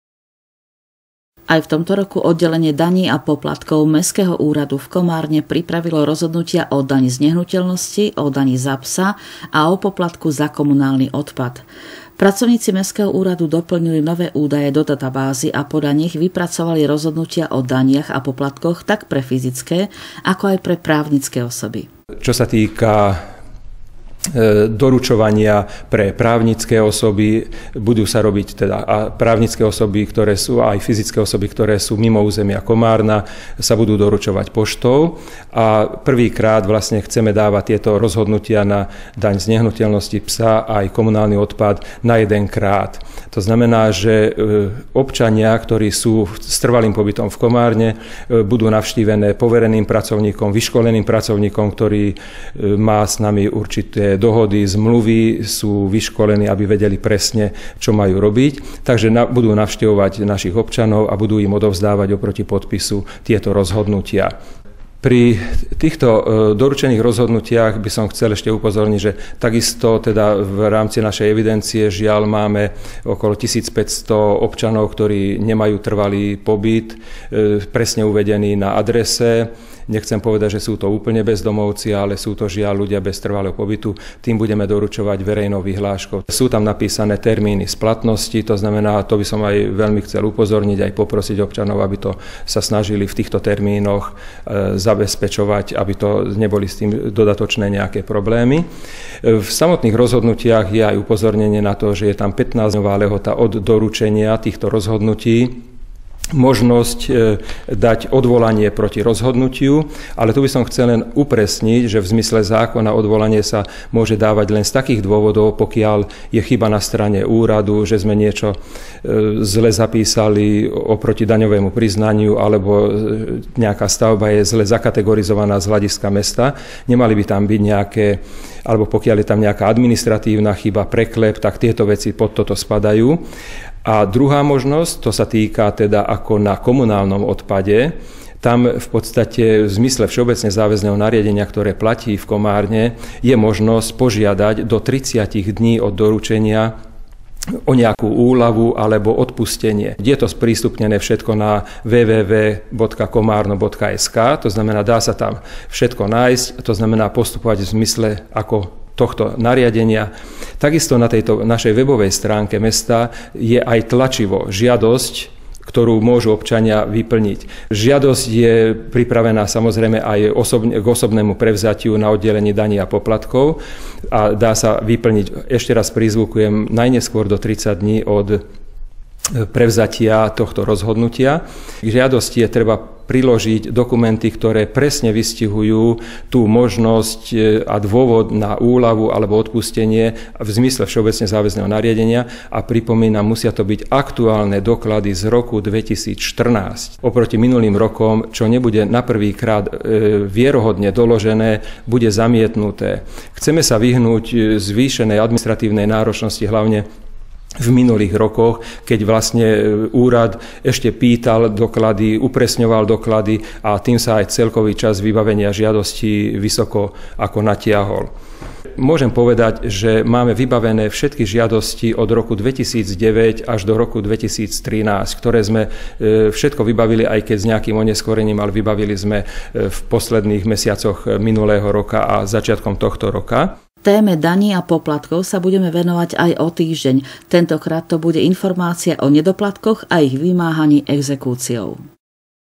Rozhodnutia o daní a popkatkov sú pripravené
Aj v tomto roku  Oddelenie daní a poplatkov Mestského úradu v Komárne pripravilo rozhodnutia o daní z nehnuteľností, o daní za psa a o poplatku za komunálny odpad.